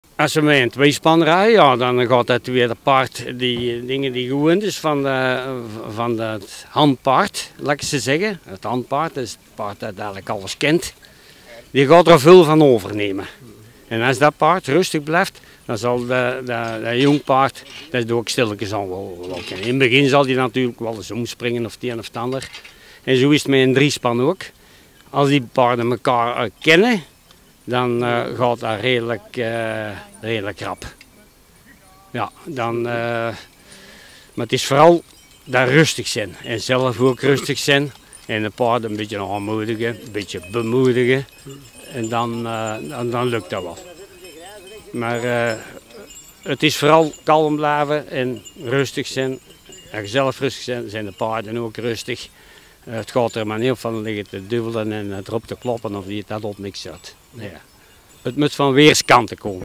Belgisch trekpaard leren ploegen
REFERENTIES ARCHIEF
… Onderwerp: Ploegen, eggen en koren zaaien voor kerststal van volgend jaar (2024) met driespan Belgische trekpaarden van Het Aangespannen Trekpaard.
… Organisatie: De vrienden van de kerststal Merksplas.
… Zaaier: Herman Verstraelen.
… Locatie: veld achter de Kapel van de Kolonie, Merksplas, Noorderkempen, provincie Antwerpen, België.
… Datum: 22 oktober 2023, 10 uur.